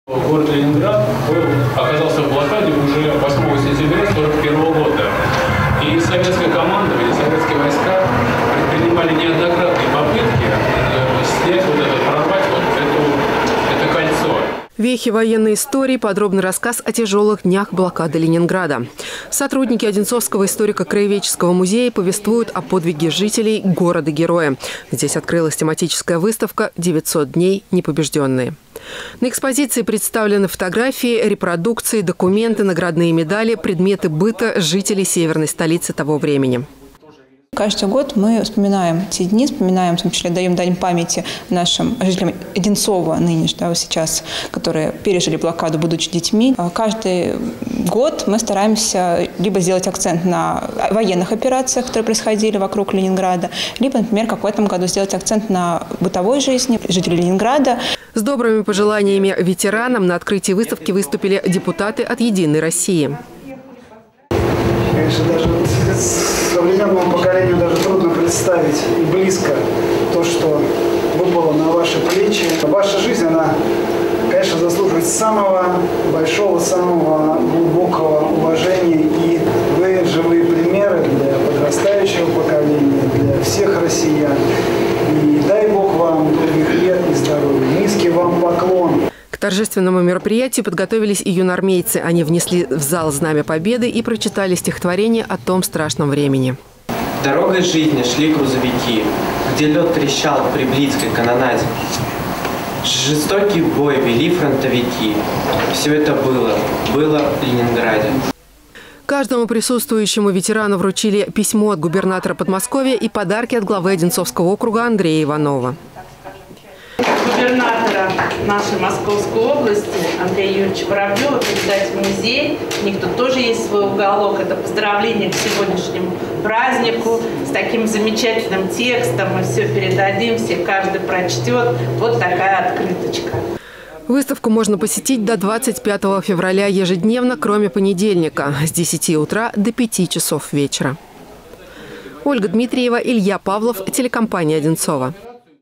Выставка «900 дней. Непобежденные»
Документы и фотографии, плакаты и предметы быта жителей блокадного Ленинграда представлены на открывшейся и Одинцовском историко-краеведческом музее выставке «900 дней. Непобеждённые». Экспозиция рассказывает о героизме бойцов Красной армии и жителей осаждённого города, чей ратный подвиг и доблестный труд разорвали блокадное кольцо.